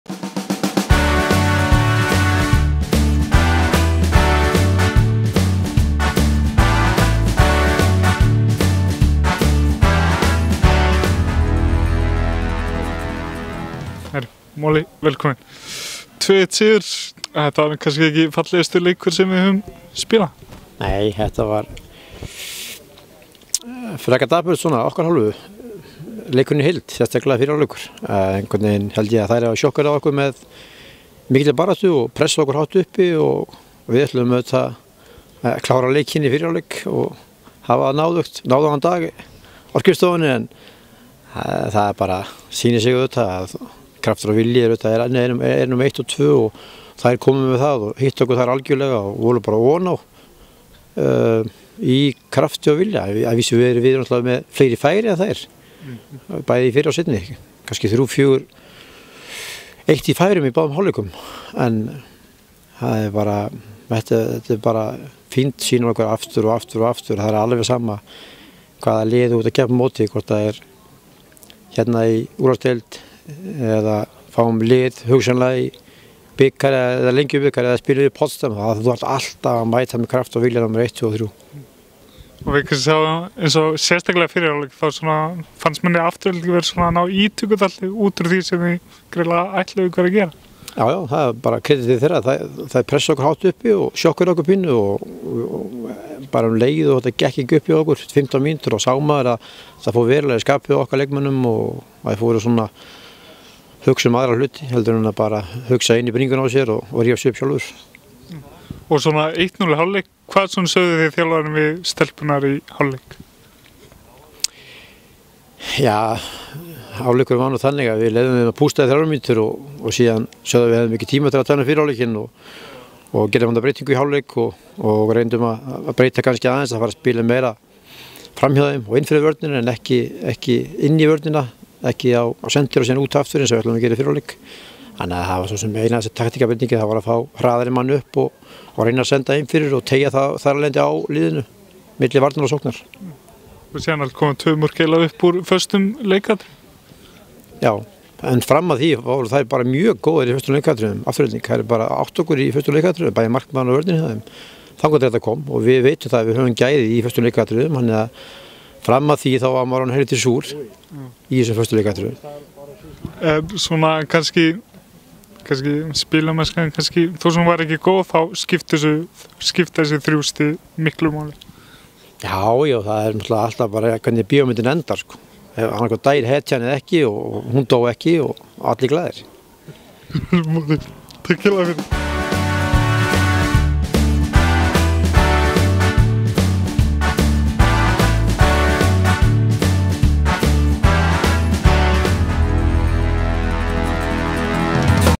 Eftir leik Afturelding: Siguróli Kristjánsson
Stelpurnar okkar unnu Aftureldingu í kvöld, 2-1 í 4. umferð Pepsi deildarinnar. Heimasiðan náði tali af Siguróla í leikslok og spurði hann út í leikinn.